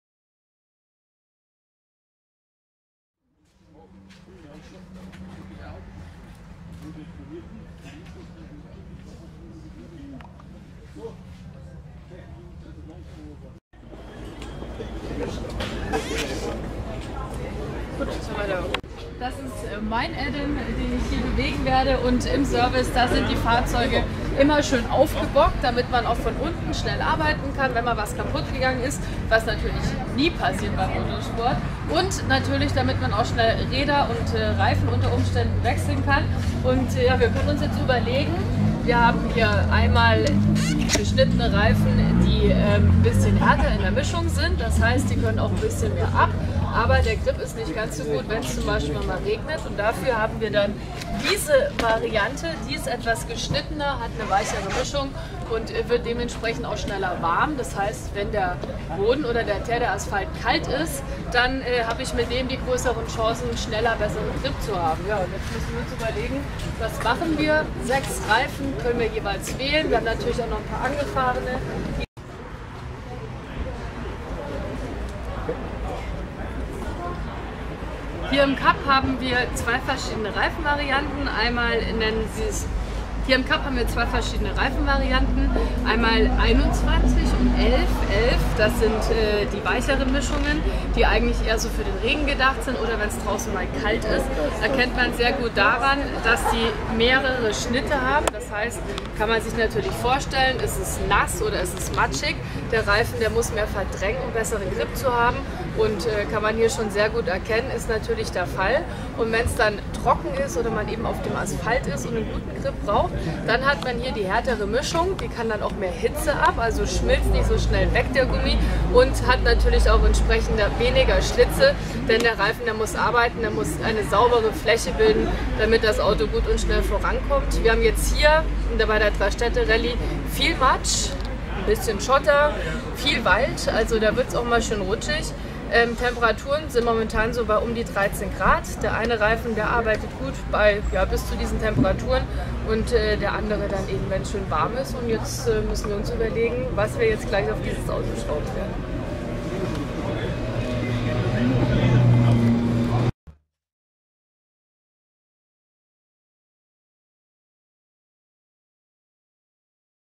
LINA VAN de MARS 3- STAEDTE- RALLYE 2014 REIFENTECHNIK [ HD ]
LINA EXPLAINS THE TIRE SOFT HARD MIX TO YOUR OPEL ADAM.